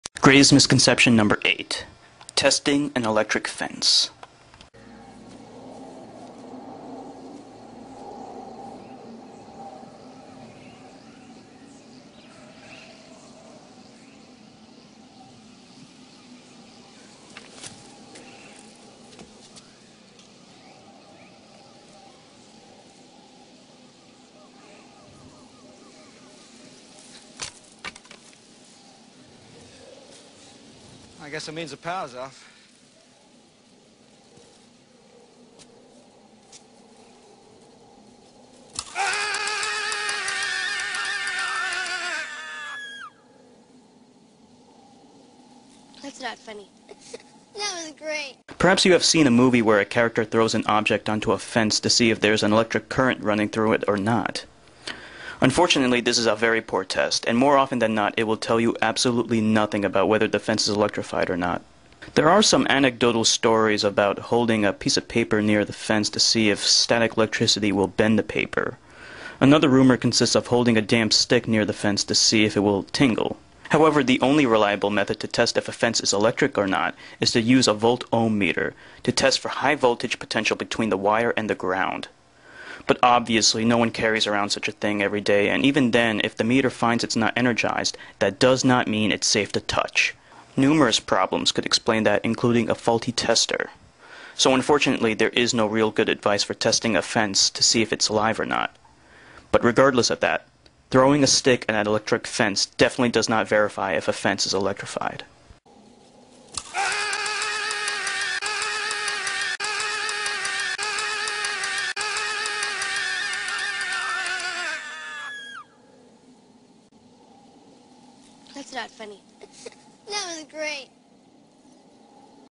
Greatest Misconception 8 Testing an Electric Fence
A common misconception hold that throwing a stick at a fence will test if it's electrified or not. Sadly throwing a stick at an electric fence tells you jack about whether it's electrified or not.